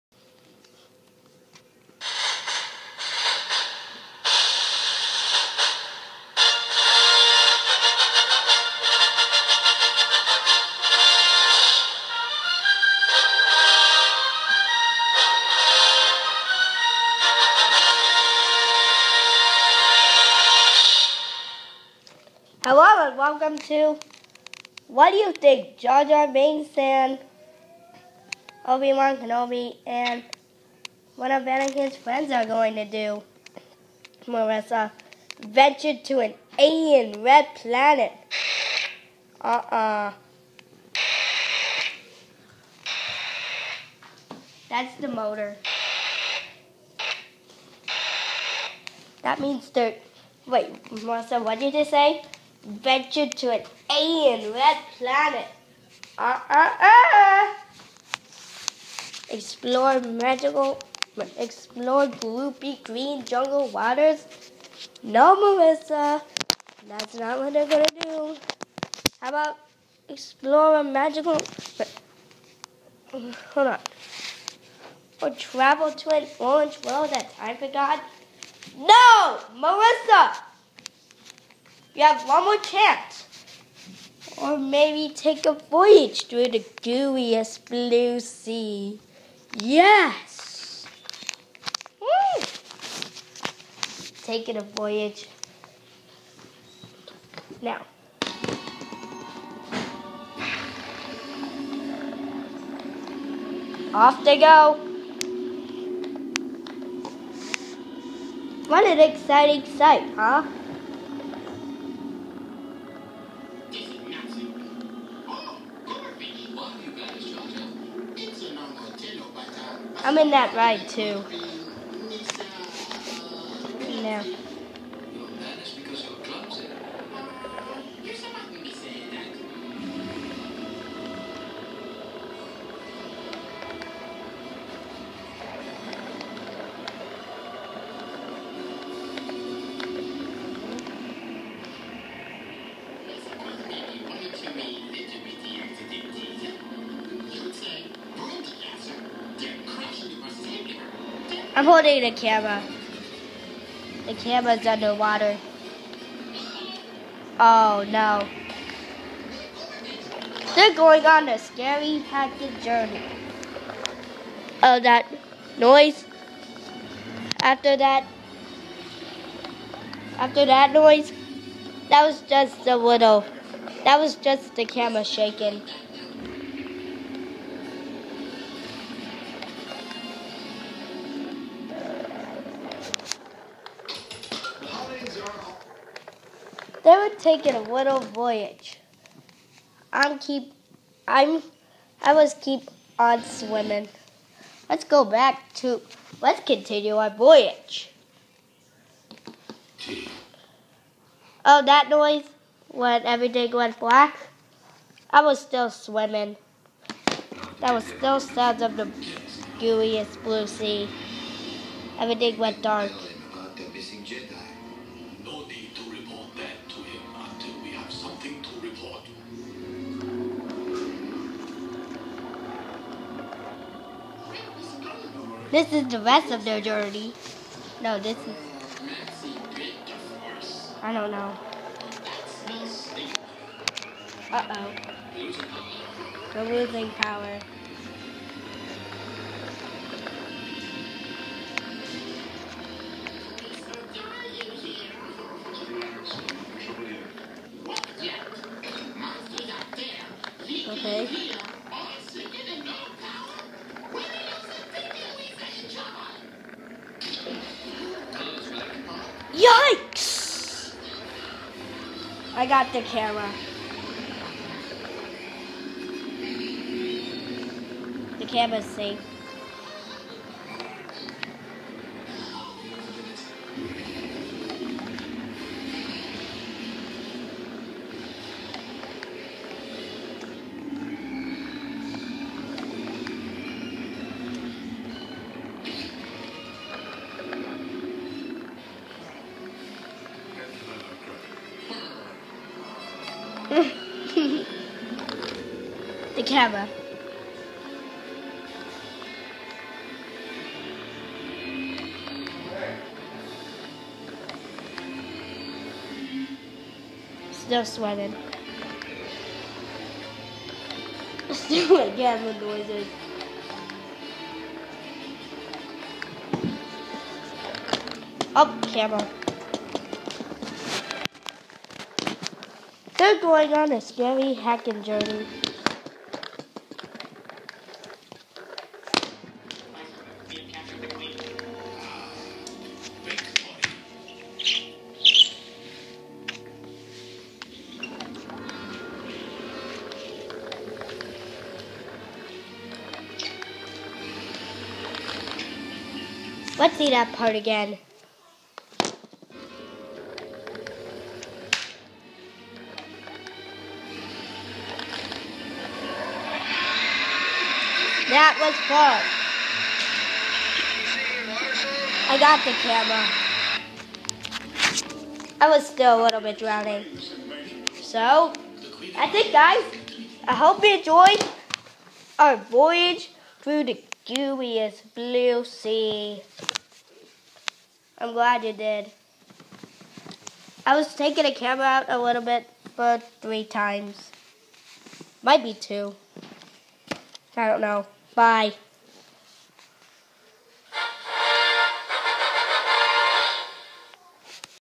Voyage through the gooiest blue sea the movie
Welcome to the underwater movie! It has underwater sounds